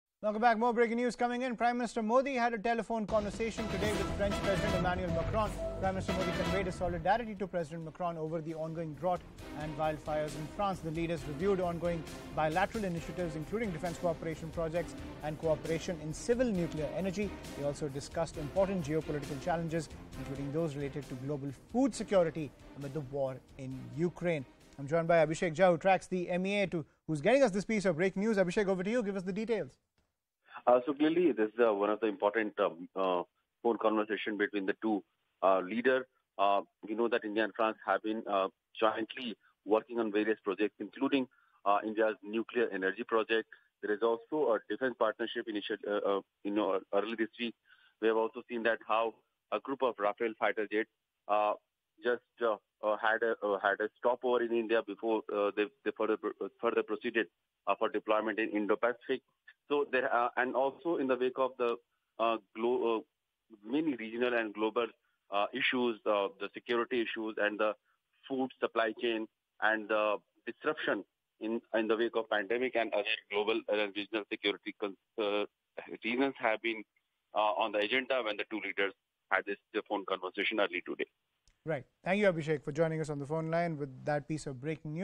PM Modi's Call With Emmanuel Macron | PM Modi News | French President | Modi Calls Macron | News18
Abhishek Jha with the details.

Join the broadcast with Ayushman Singh Jamwal